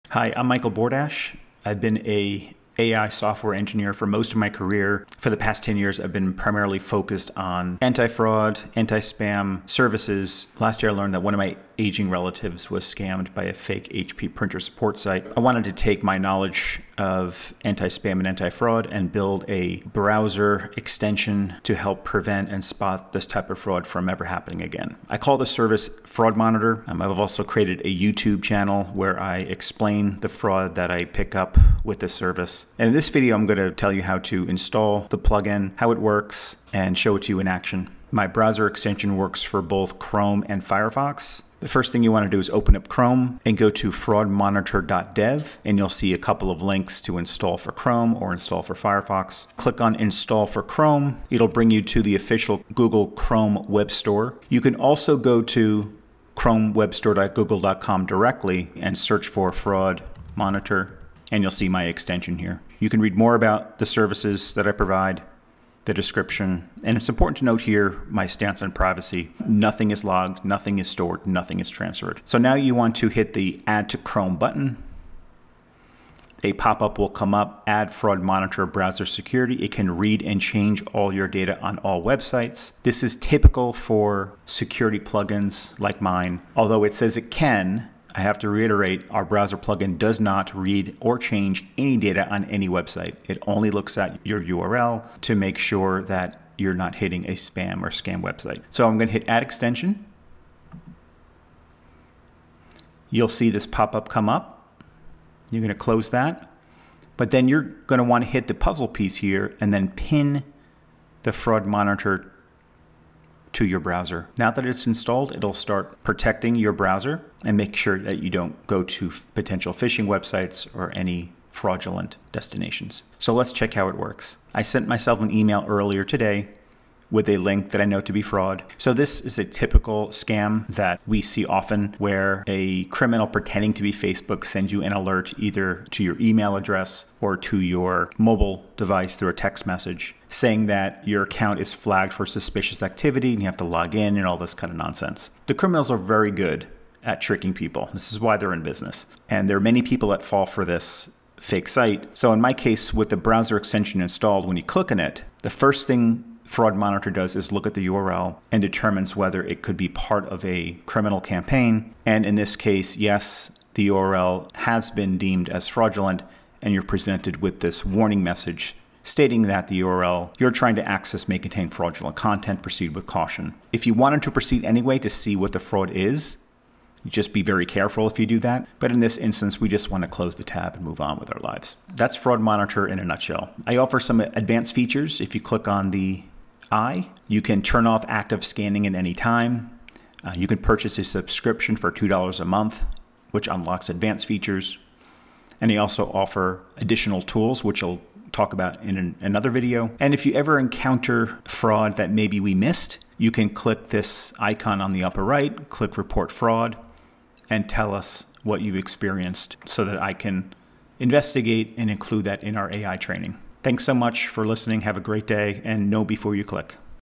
Fraud Monitor Intro and Browser Extension Installation
Welcome to Fraud Monitor! In this video, we’ll introduce you to Fraud Monitor – your go-to tool for staying safe online – and walk you through the quick and easy steps to install our Chrome extension. Whether you're shopping, browsing, or just exploring the web, Fraud Monitor helps you spot scams and protect yourself from fraud in real time. Stay secure and Know Before You Click!